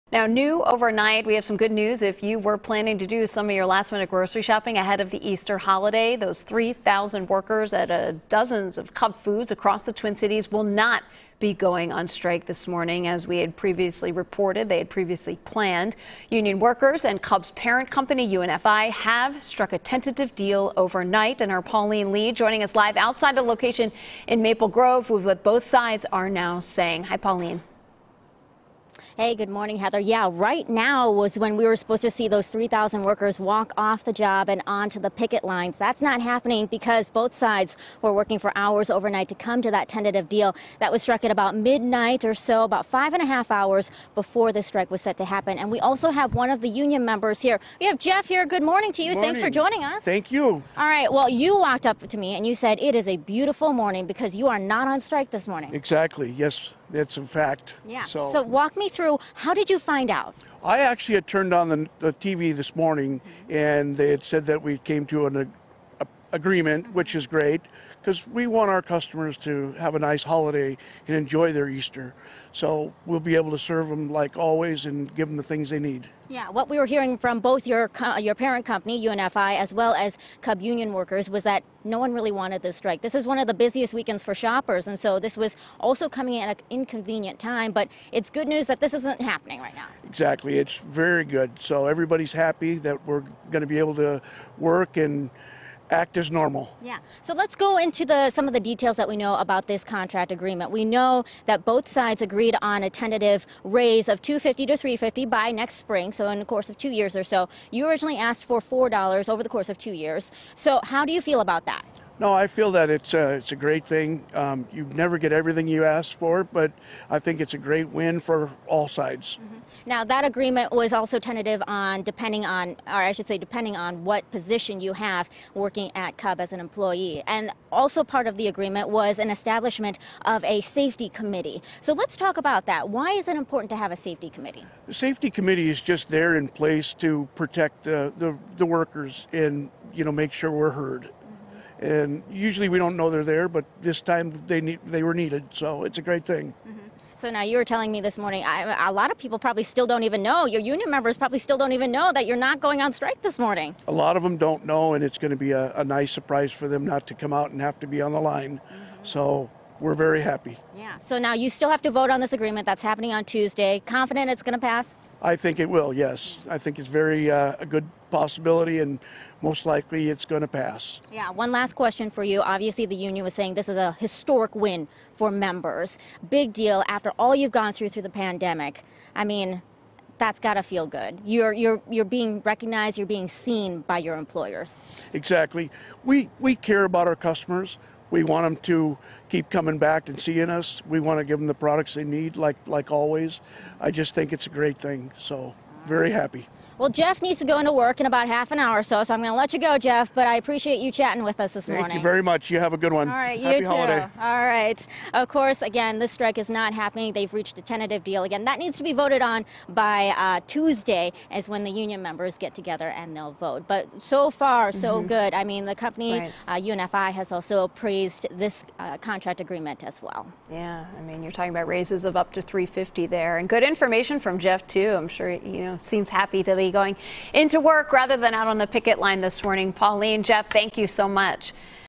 Tentative agreement made to avoid strike at 33 Cub Foods locations Friday
Cub Foods' parent company UNFI has struck a tentative deal with its 3,000 union workers, avoiding a strike that was set to start Friday morning.